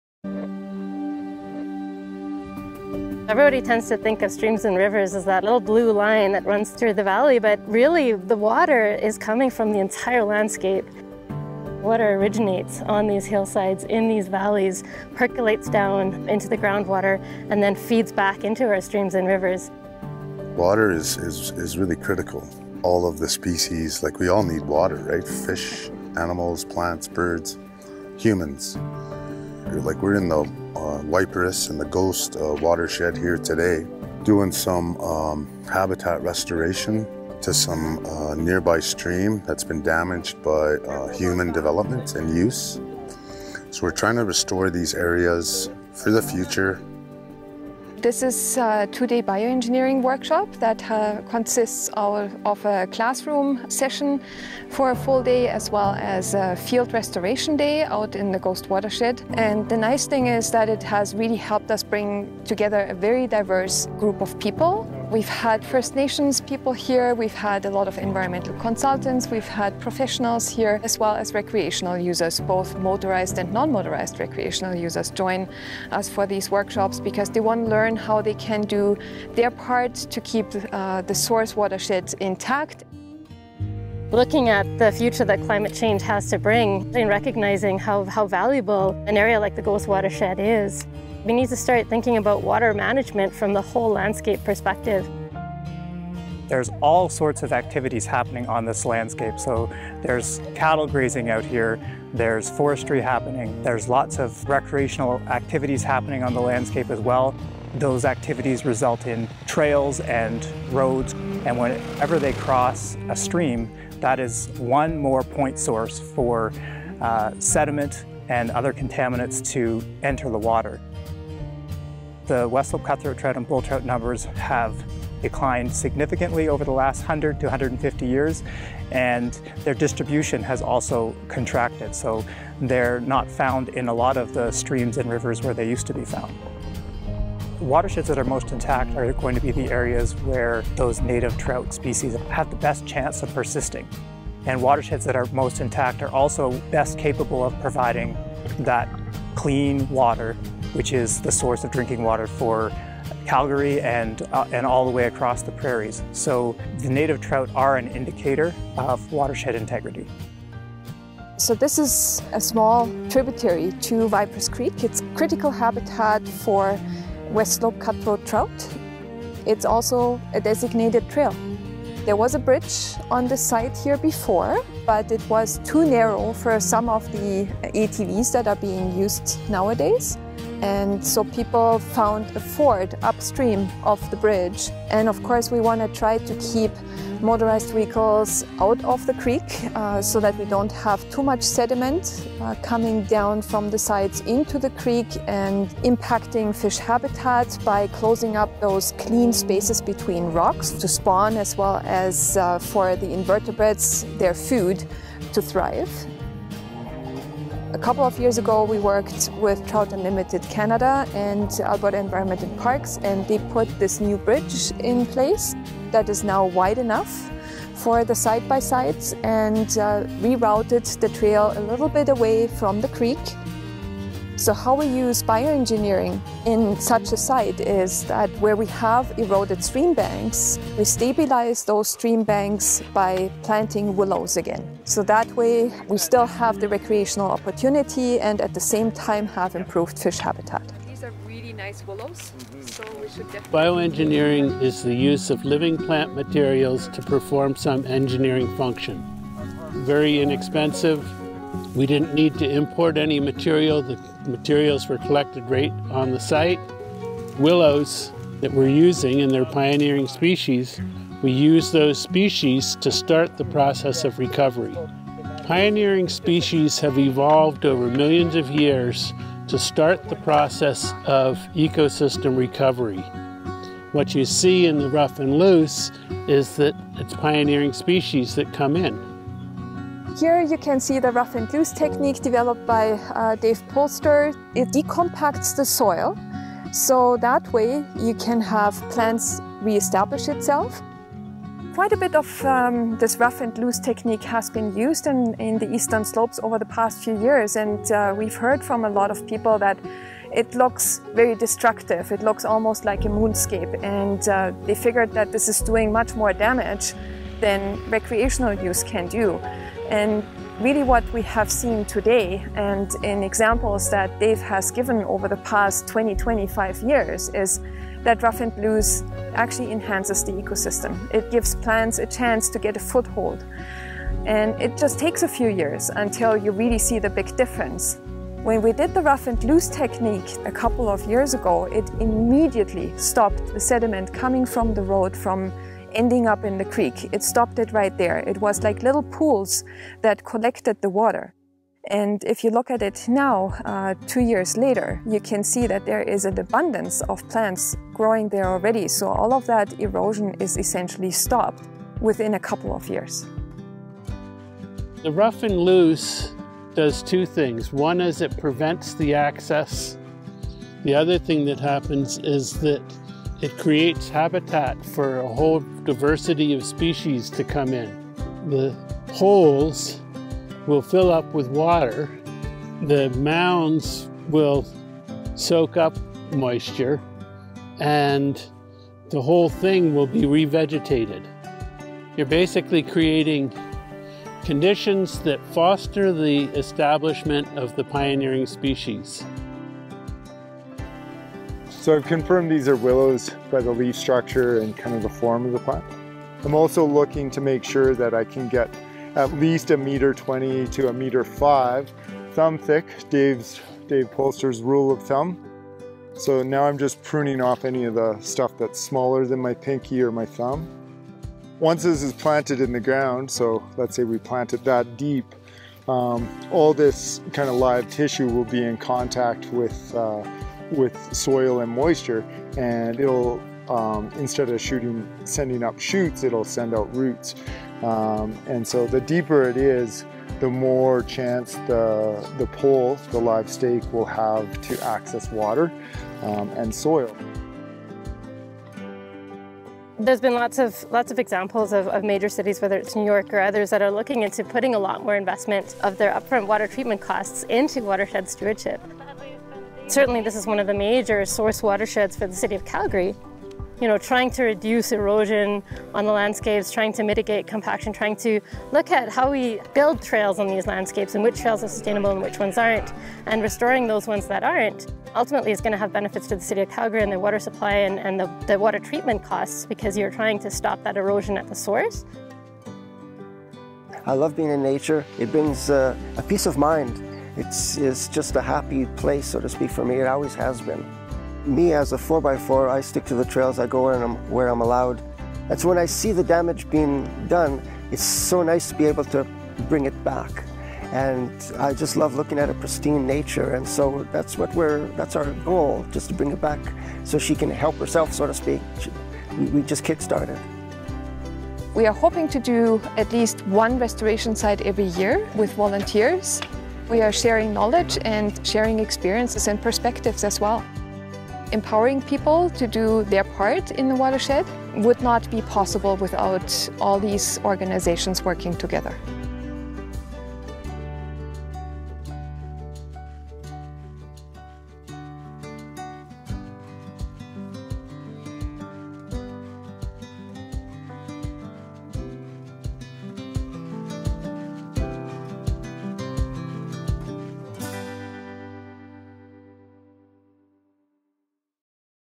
Working Together to Restore our Watersheds
The Ghost Watershed Alliance Society (GWAS) invites you to watch their latest video project, "Working Together to Restore our Watersheds."

Watch as volunteers learn from David F. Polster, a plant ecologist and experienced restoration specialist, as he demonstrates and explains effective bioengineering techniques. See and hear people speak about the methods and purpose behind the restoration work and why they are engaged in protecting Calgary's source water area.